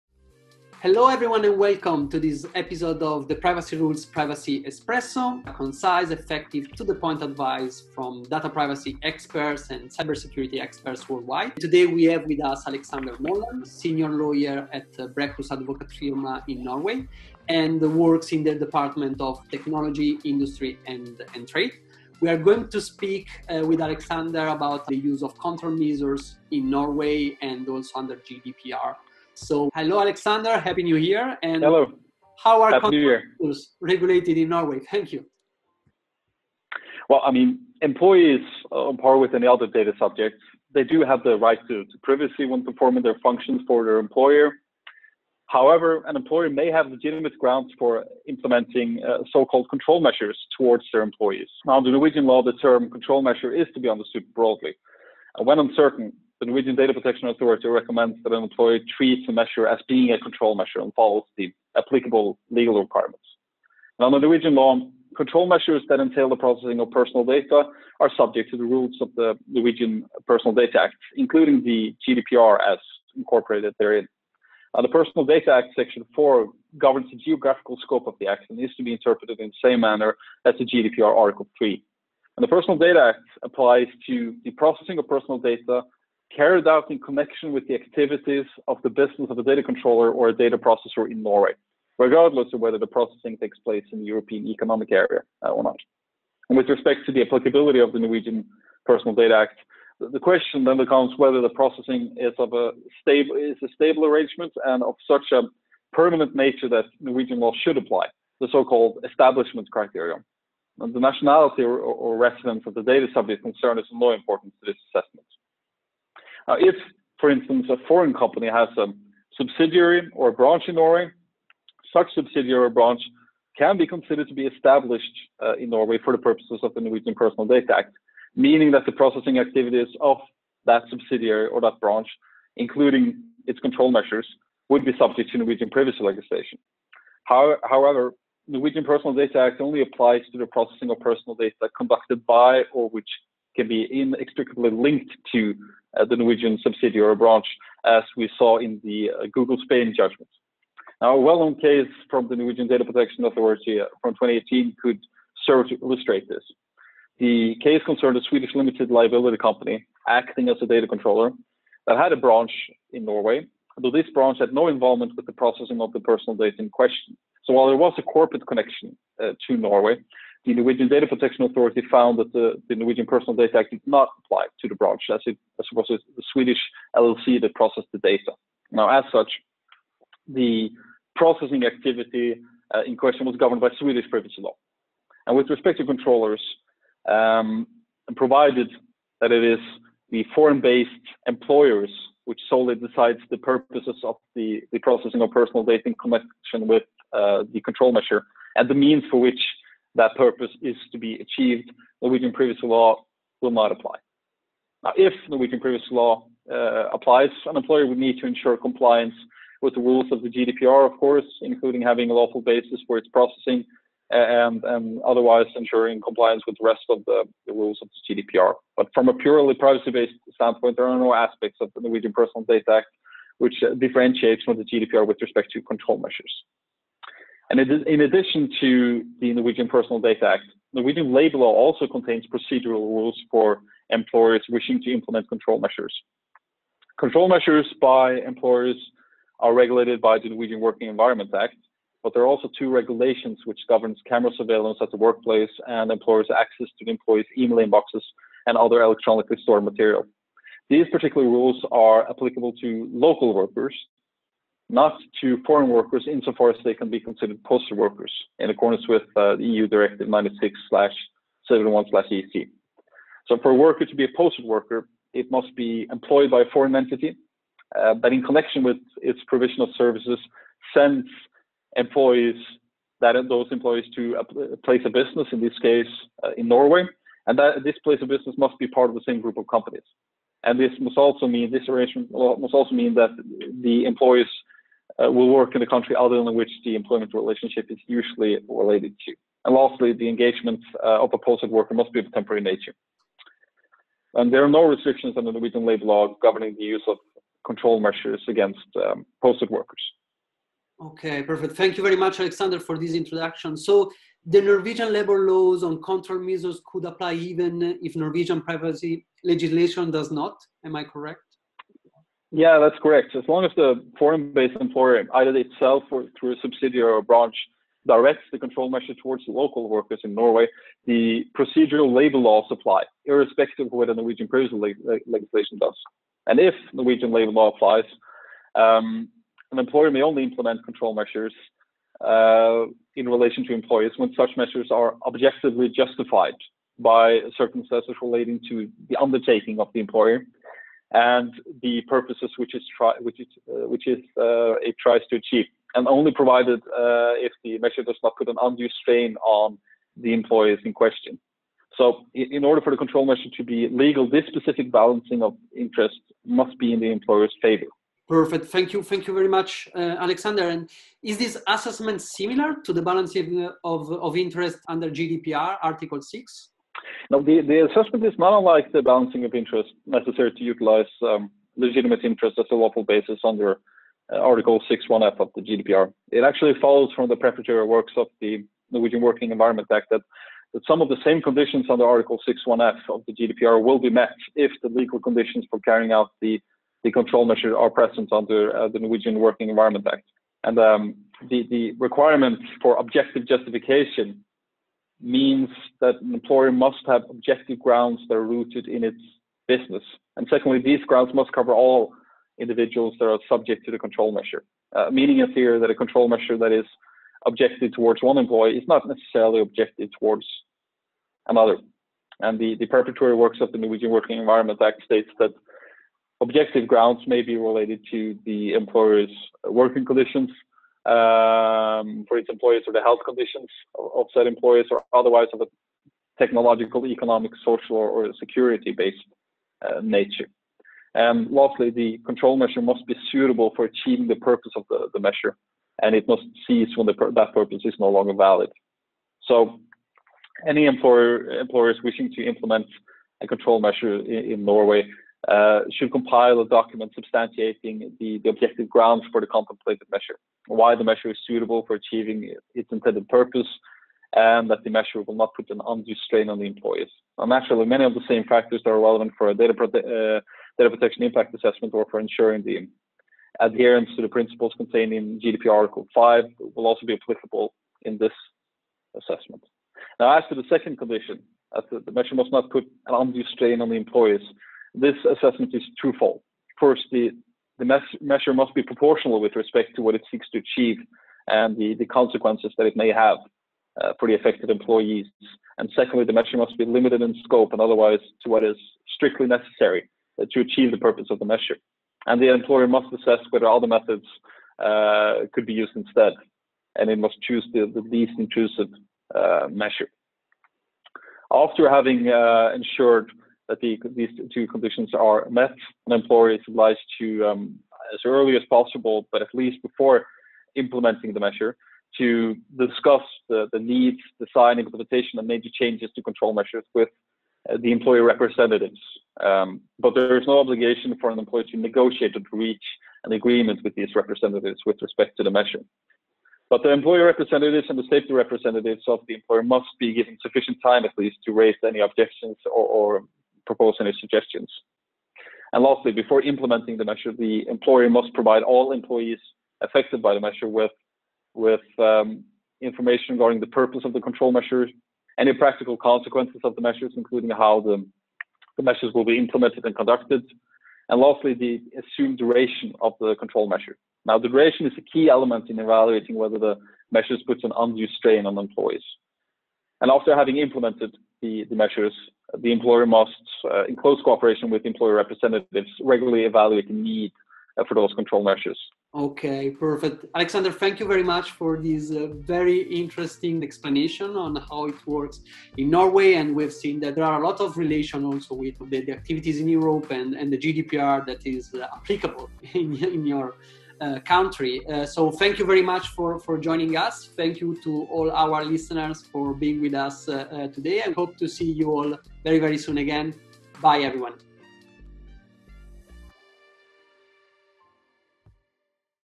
Privacy espresso series - The do's and don'ts of employer-conducted control measures in Norway
With the ever-increasing presence of technology in the workplace and the risks associated with the use of digital tools, information sharing and employees working from home, control measures in Norway are on the rise. Such, measures, ranging from access controls to GPS tracking, are subject to the GDPR as incorporated into the Norwegian Personal Data Act. Furthermore, the Norwegian Working Environment Act and its appurtenant regulations impose several procedural obligations on employers wishing to implement control measures in the workplace. Failure to comply with these obligations could not only expose the employer to potential liability and loss of reputation, but also render any evidence gathered through the measure inadmissible before the Norwegian courts.

Our Norwegian expert, Alexander Mollan from Brækhus Advokatfirma DA, delves into this complicated patchwork of laws in order to provide you with some practical tips on how to avoid any risk dealing with this sector.